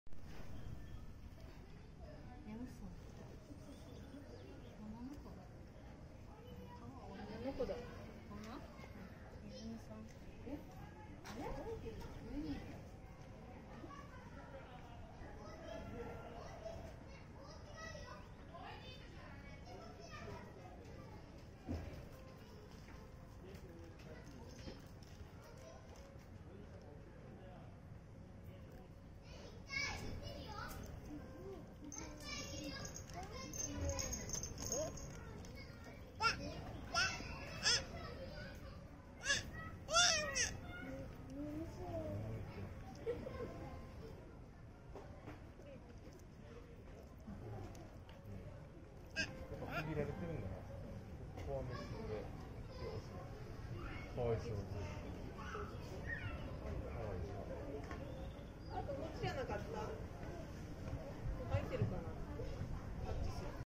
沖縄こどもの国★春時雨♥⑭♥ホワイトライオン・白獅子・white_Lion・リズムちゃん・感じろ四字熟語：諸行無常・しょぎょうむじょう・・・Okinawa Zoo & Museum(20210213)
オス　アルーシャ　２０１０年１０月２３日生
メス　カノ　２０１２年２月１１日生
オス　セラム　２０１６年１１月１４日生　ホワイトライオン
メス　リズム　２０１８年８月８日生　ホワイトライオン

ライオン（Panthera leo） は、食肉目ネコ科ヒョウ属に分類される食肉類の哺乳類。
別名はシシ（獅子）。